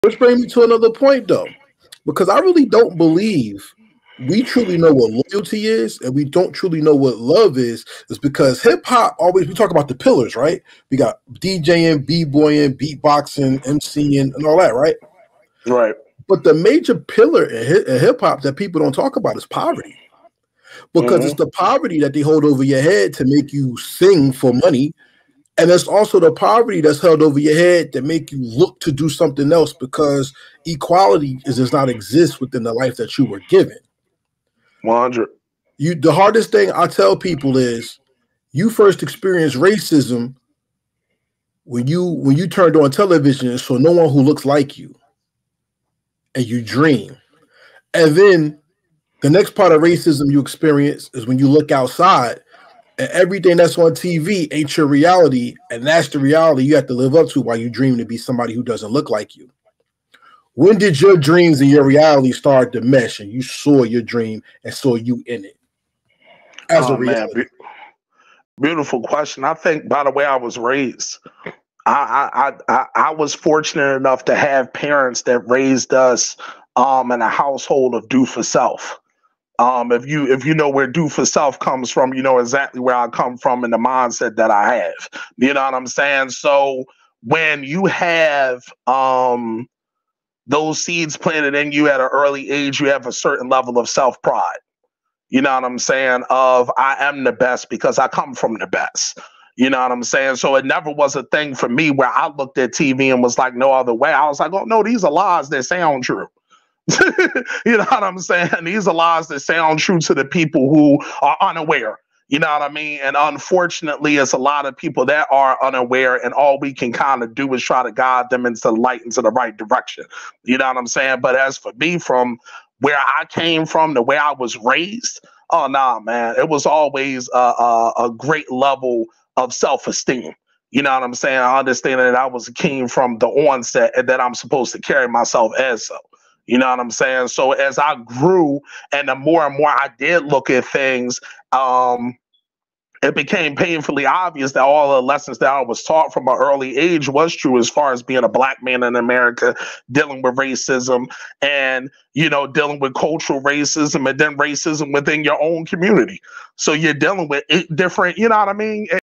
The Underrated Pillar of Hip Hop: Poverty and its Impact on Dreams
Explore the often overlooked pillar of poverty in hip hop and its influence on artists' dreams and realities. This thought-provoking video delves into the challenges of inequality, racism, and self-pride in the pursuit of success.